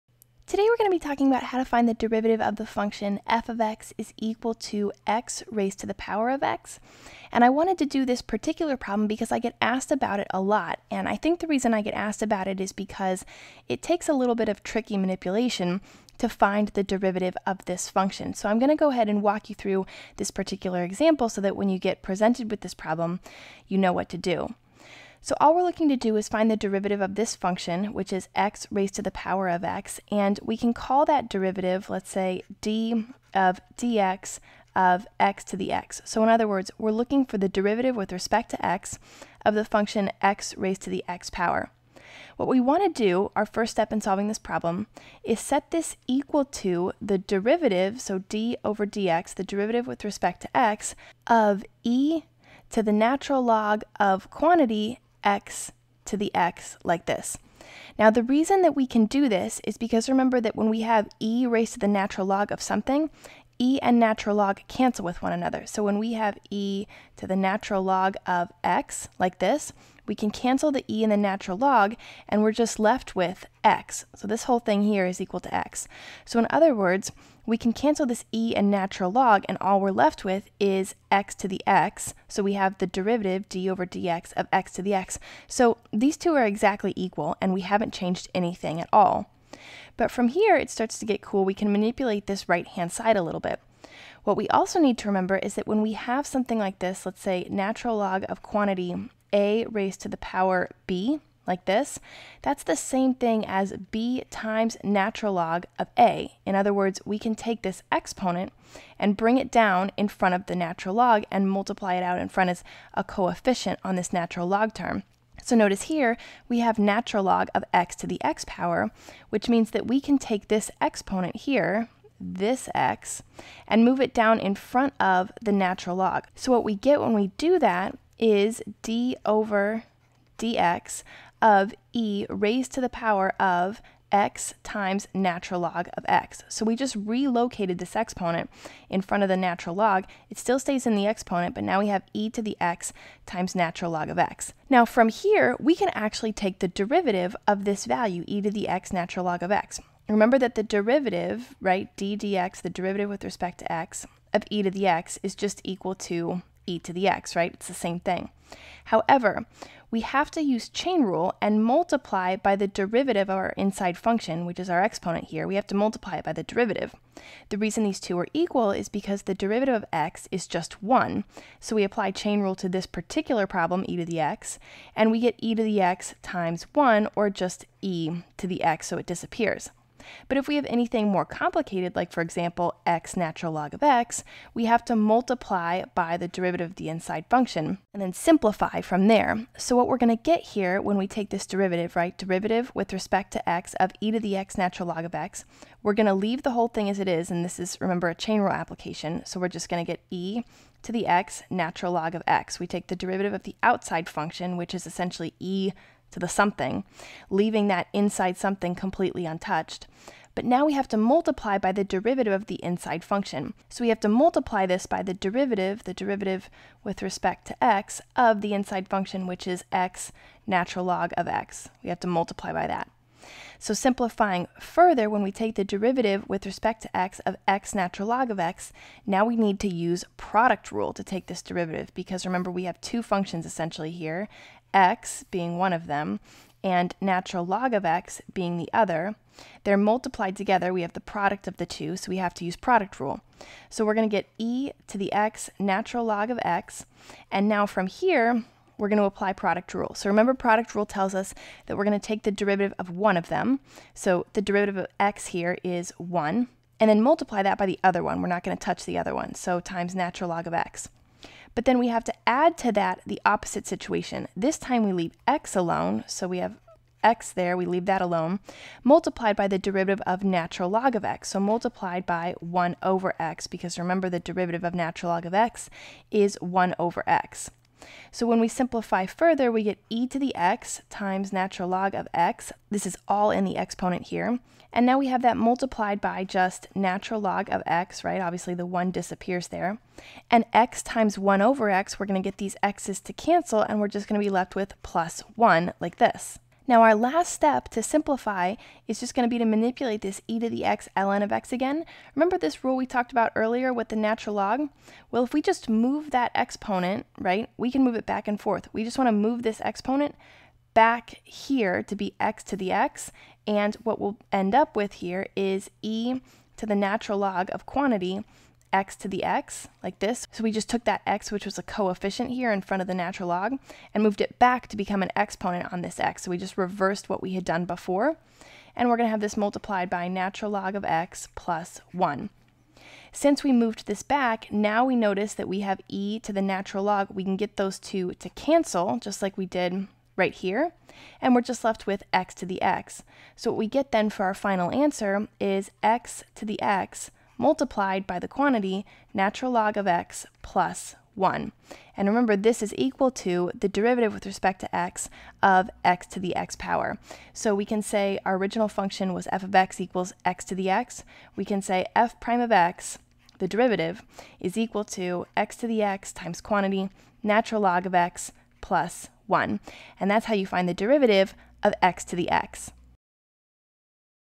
Derivative of x^x (KristaKingMath)
Learn how to find the derivative of x^x using a proof.